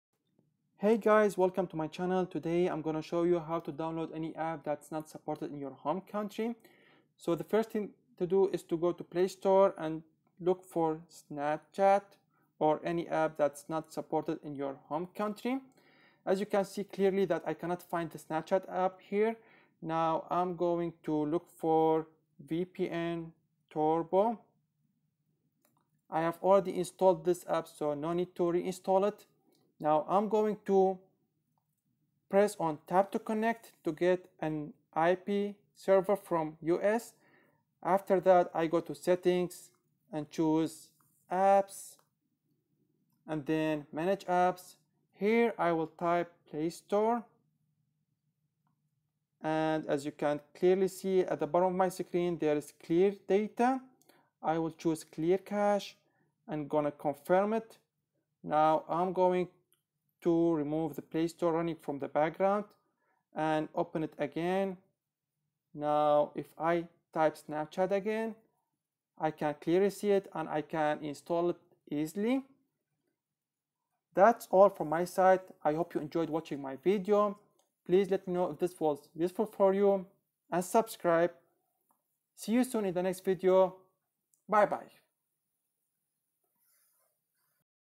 How to Fix "This item isn't available in your country"
How to Fix "This item isn't available in your country" on Android (Play Store) and install any application that is not supported in your country.